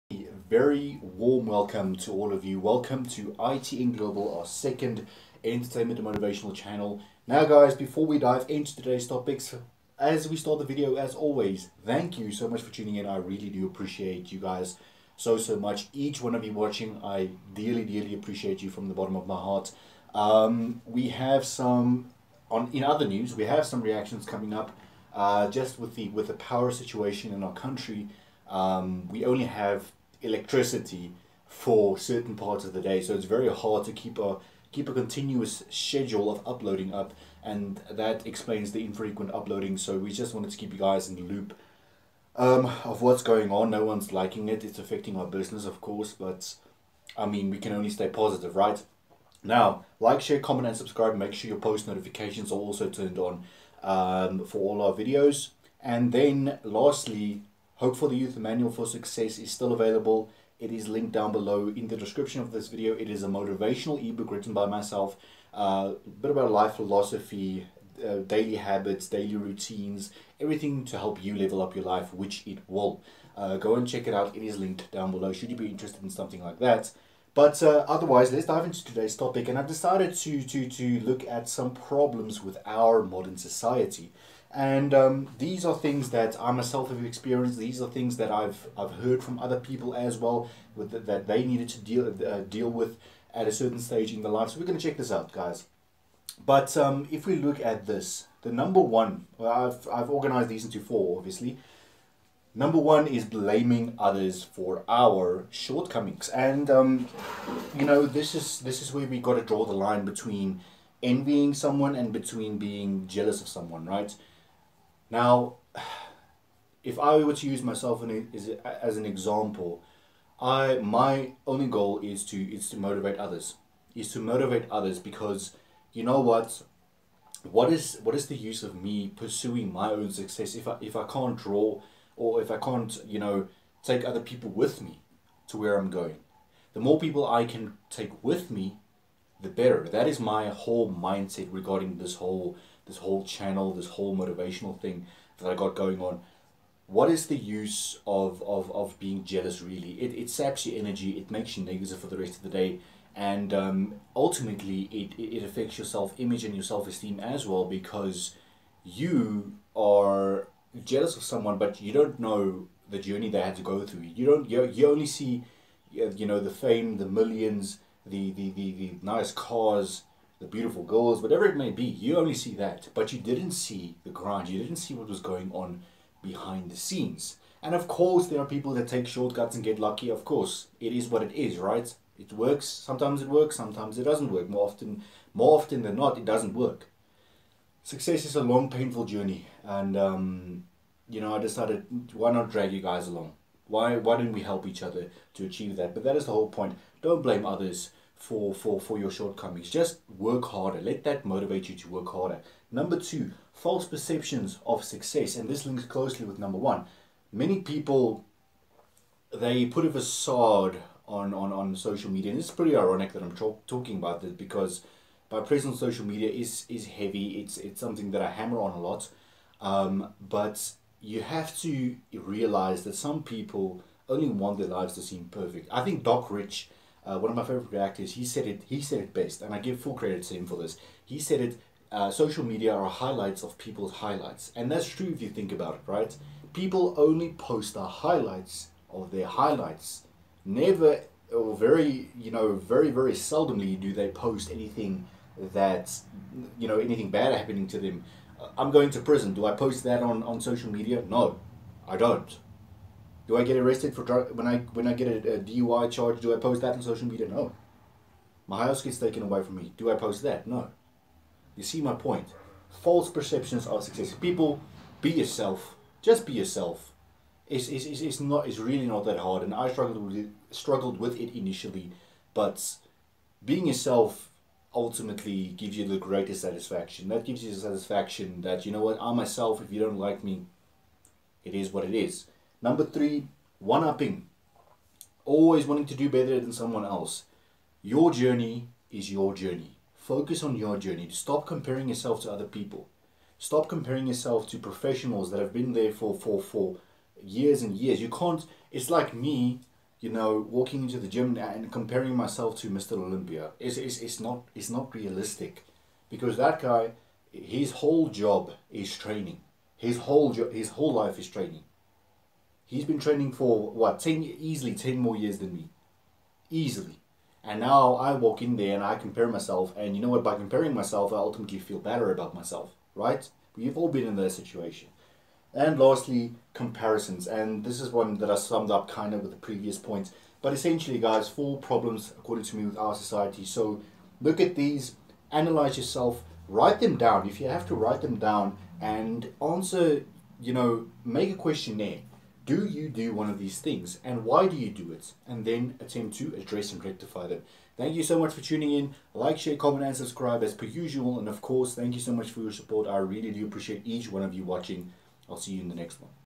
4 BIGGEST PROBLEMS IN OUR SOCIETY
Our society is seemingly riddled with problems that lead to fragility, superficiality and violence.  Let's take a look at 4 of these problems!
The Ultimate Millionaire-Mind shift course to help you get your life on track